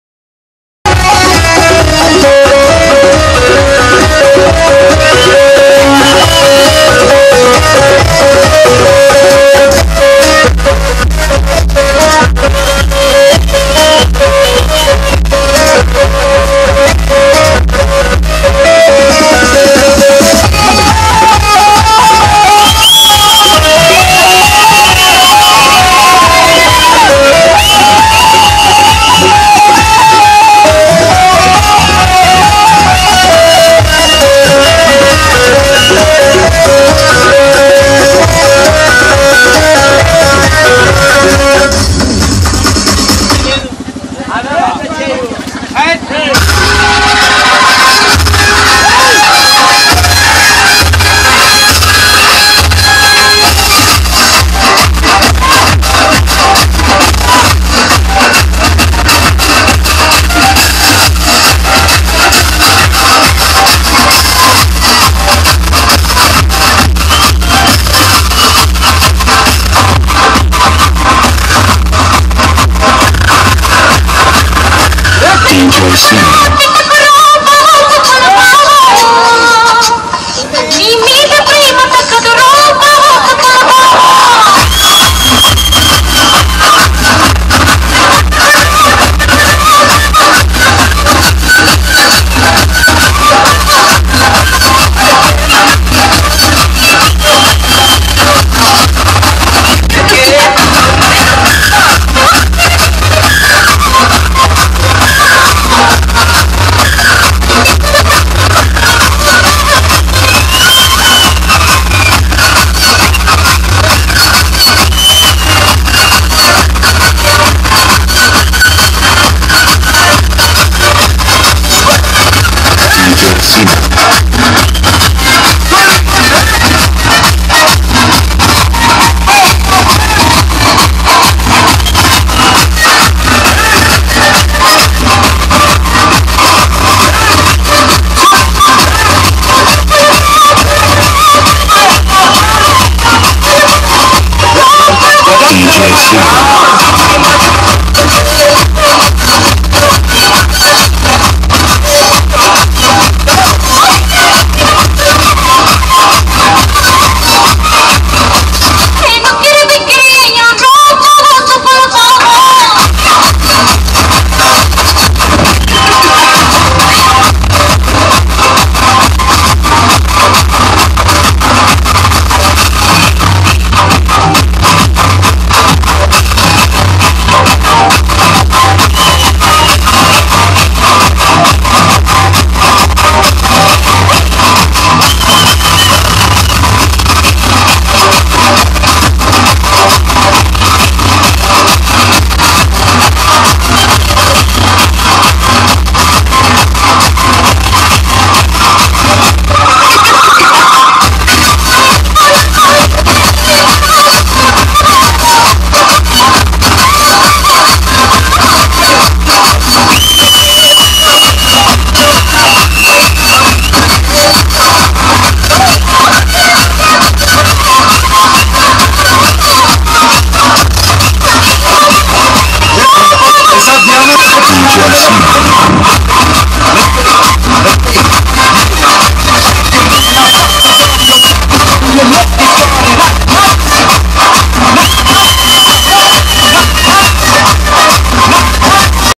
Anil bahttu famous snake dance
Babai marriage I will performance snake dnace bahhtu thanda (m) parvathagiri( d)warangal